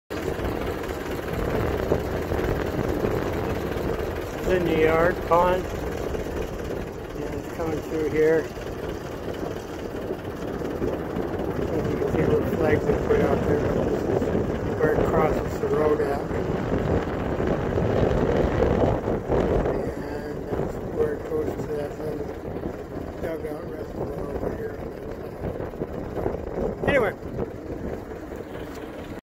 Homestead waterflow
Road crossing flow 2 May 2020